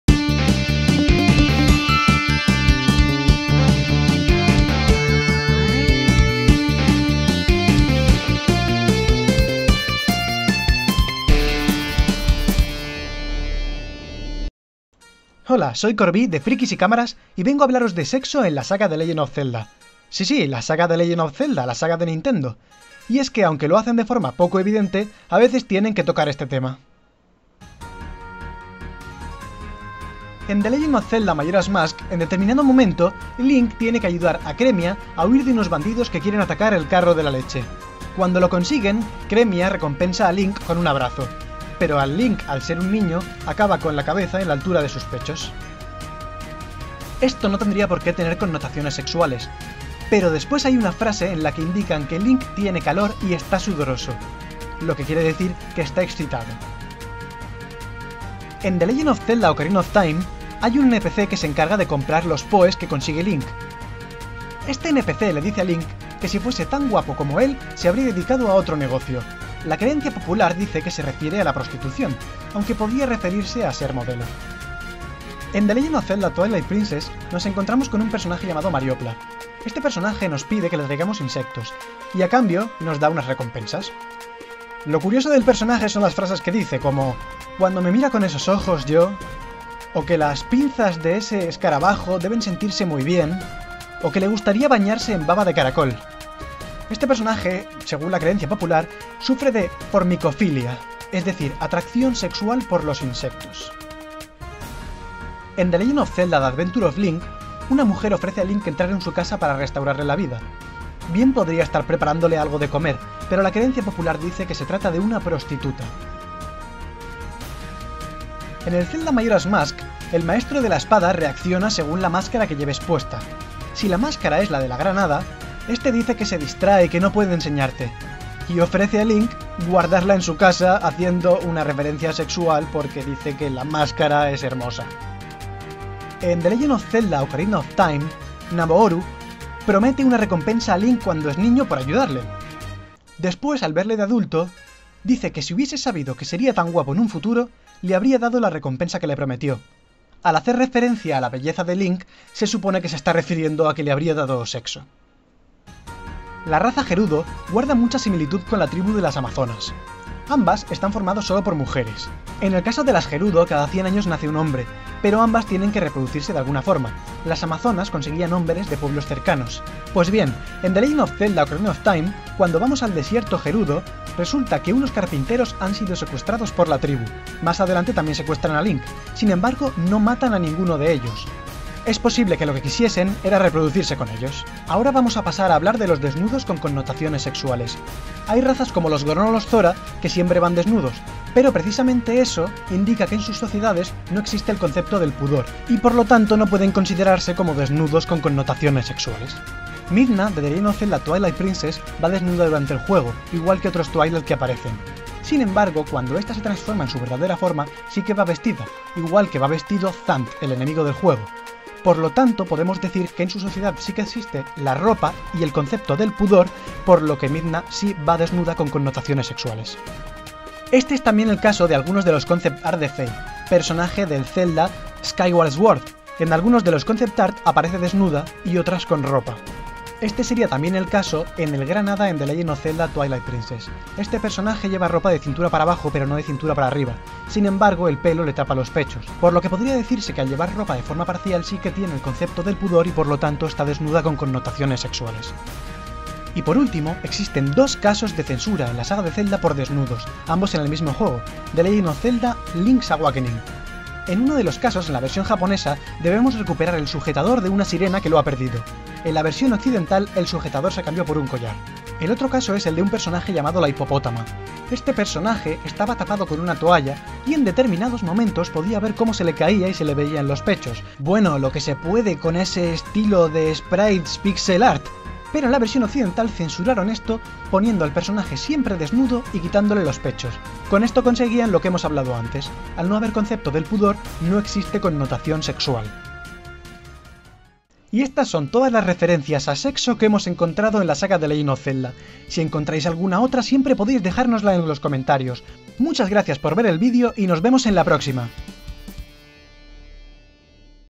Sexo en la saga Zelda | Artículo
En esta ocasión os traémos un video hablando sobre el sexo y los desnudos en la saga The Legend of Zelda.

Es evidente que no se trata de algo explícito, pero aquí te explicamos todos los detalles al respecto.